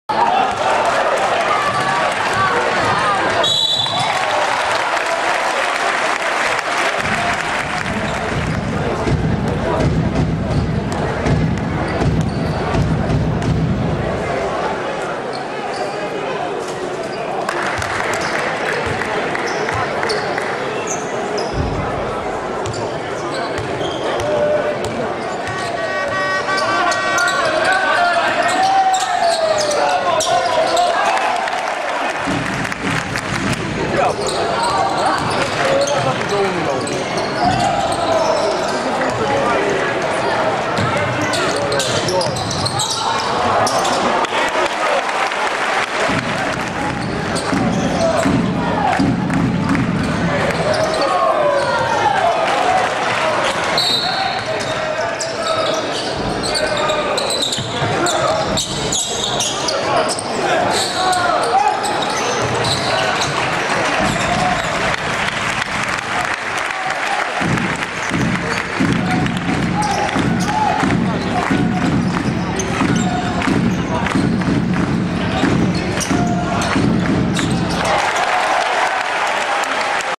Rezumat _U_ Mobitelco - Timba Timisoara 09.11.2012
Echipa noastra a trecut de Timba Timisoara, scor 71-60, in cadrul celei de-a 7-a etape a Ligii Nationale. Victoria i-a fost dedicata lui Tyler Morris, cel care a renuntat la cariera de baschetbalist in urma accidentarilor repetate.